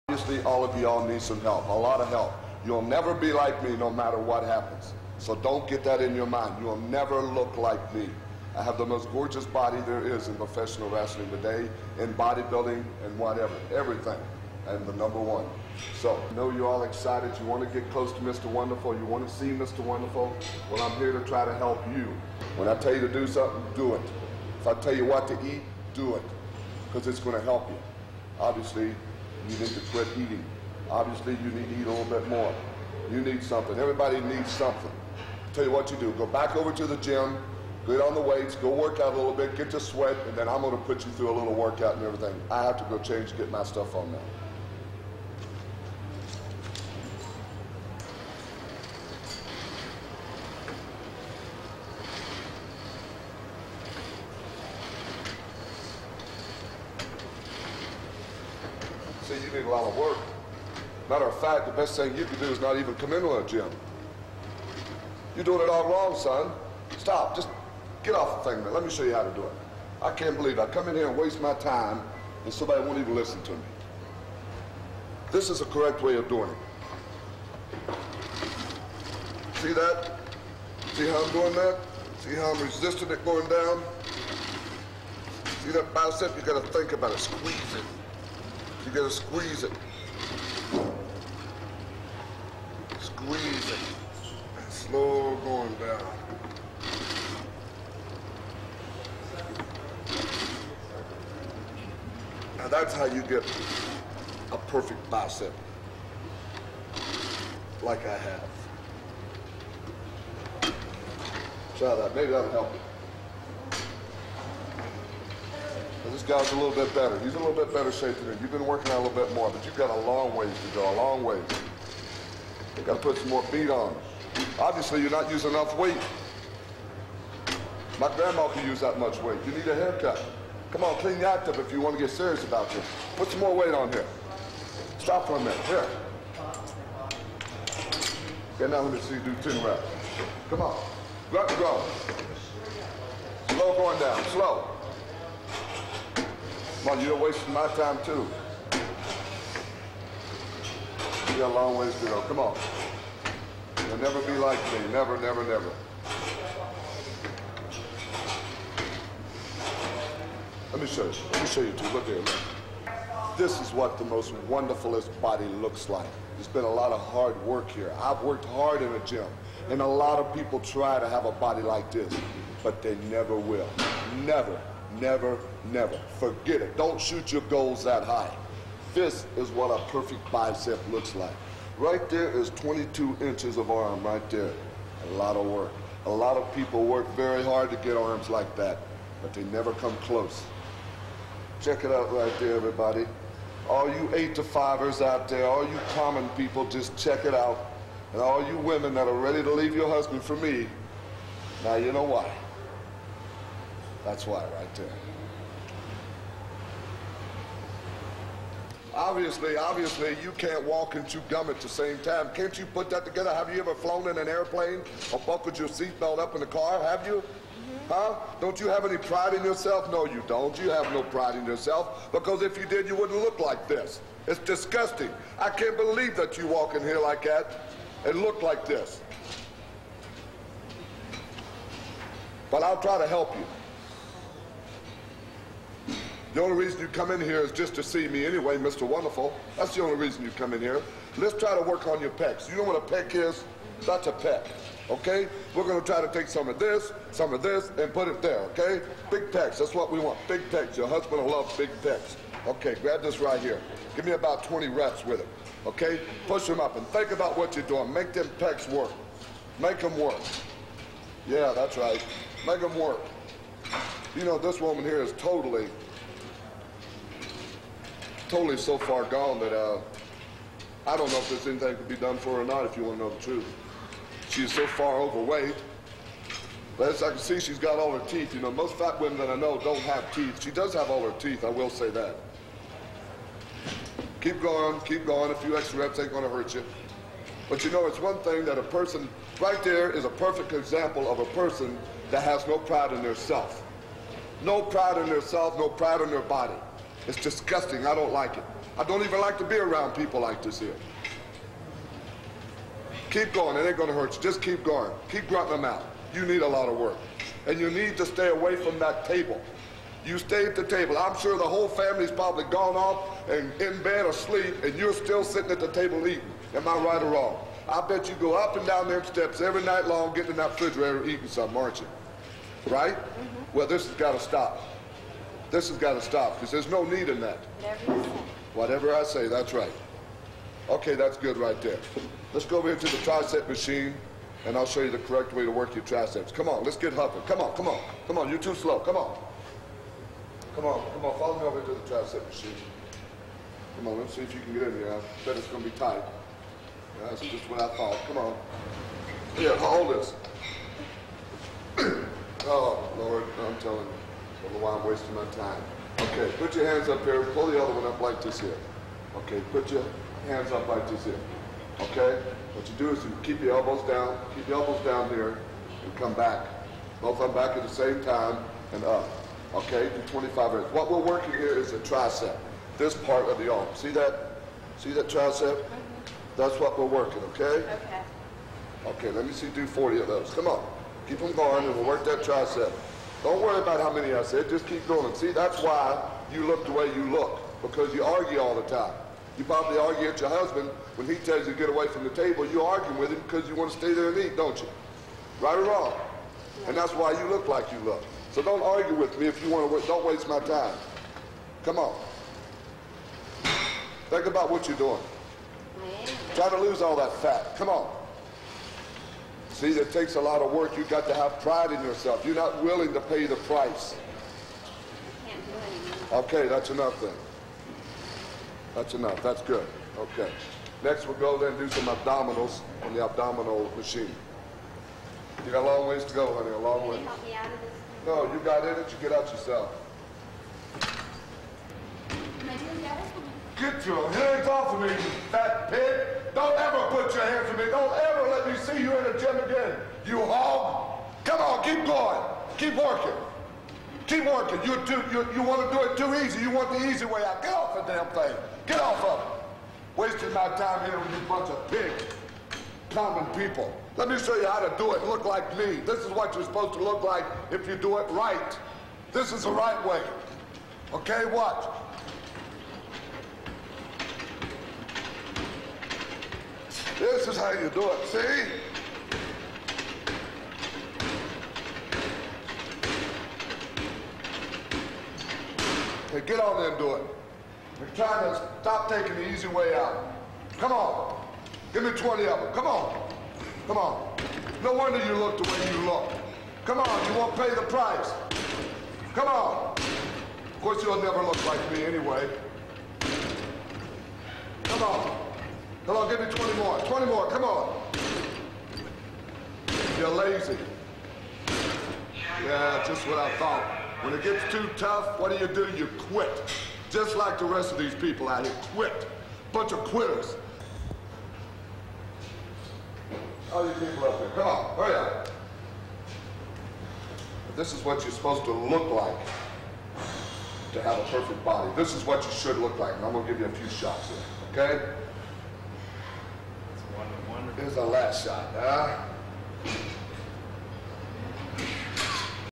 "Mr Wonderful" Paul Orndorff at the Gym (1984)
MrWonderful Paul Orndorff shows people how to get in shape at the local gym 80s WWF promo